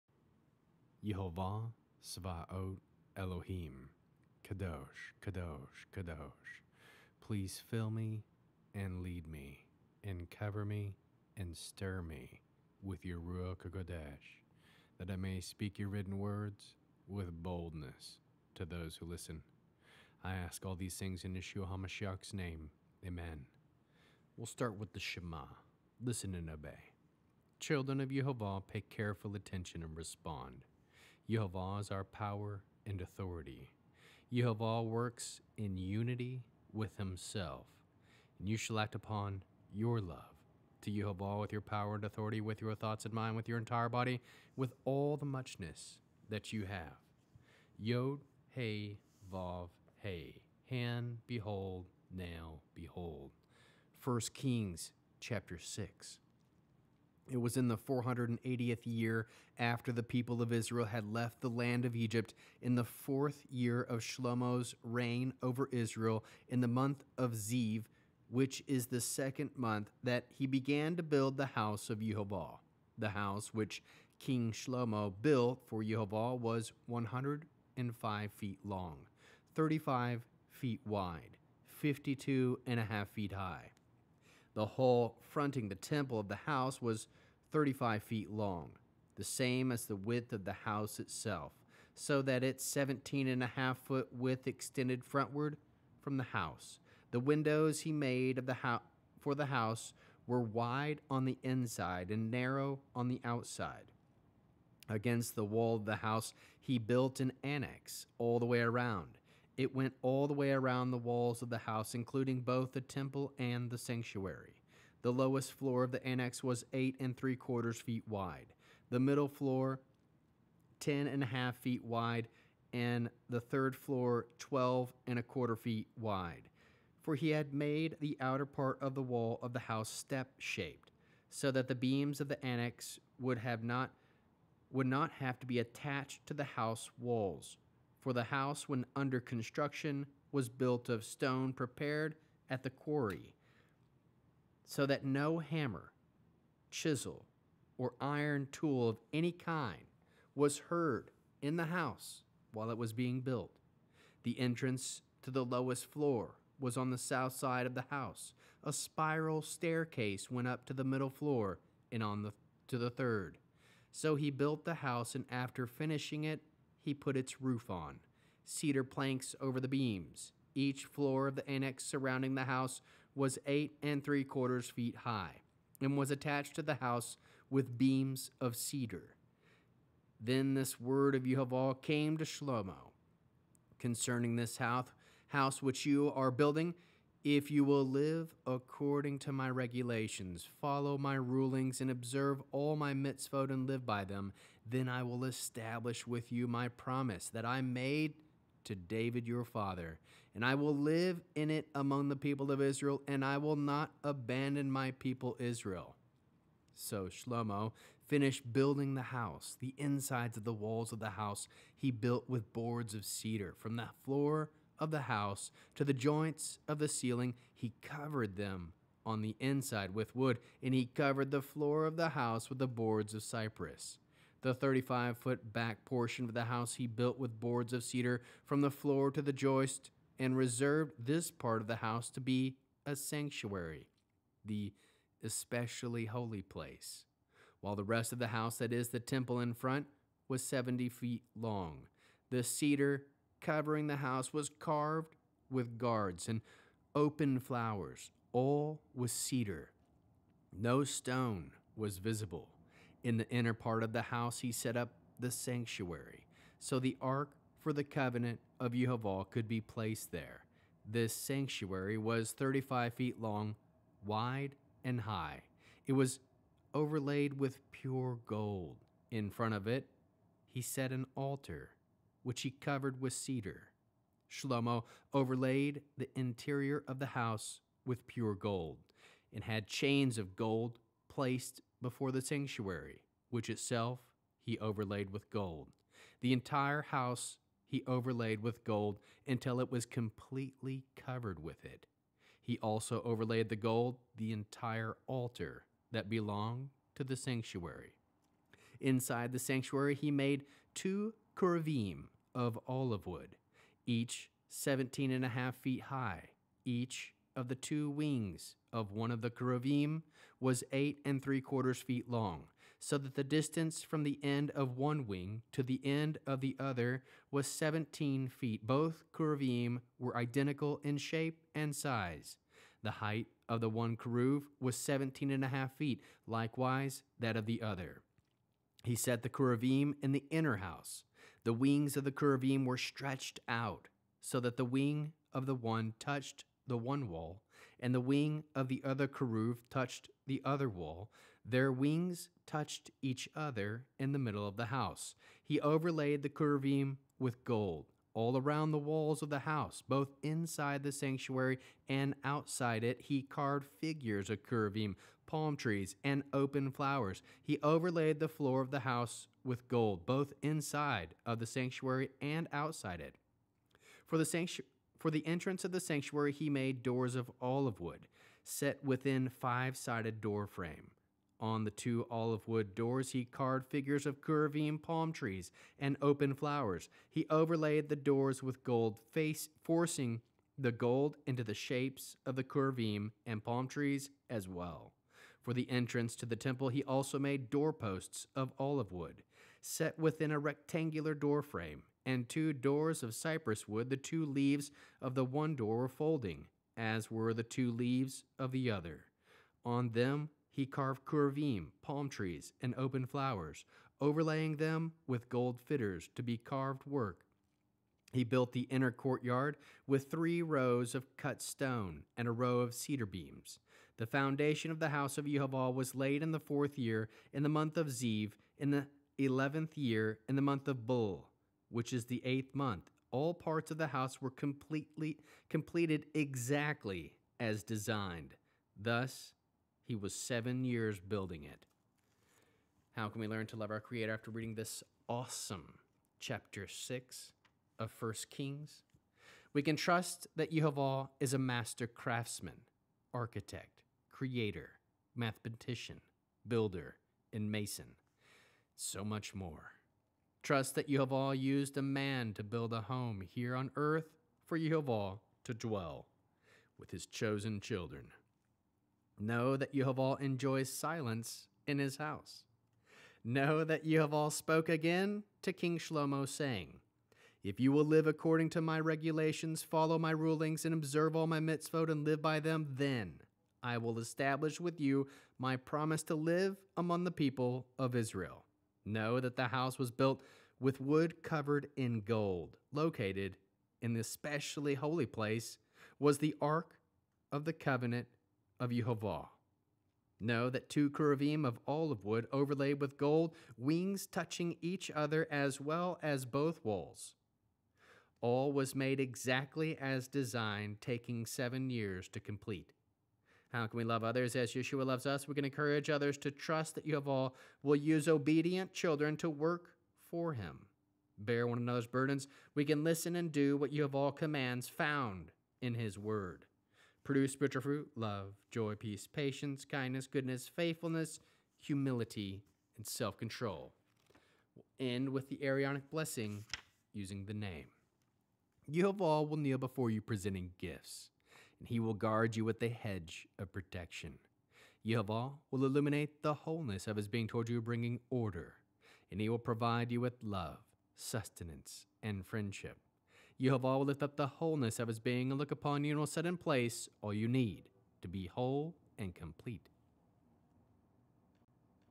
1 Kings 6 • Day 1,772 - listen and do what God יהוה commands found in His written Word ❤️
Complete Hebrew Bible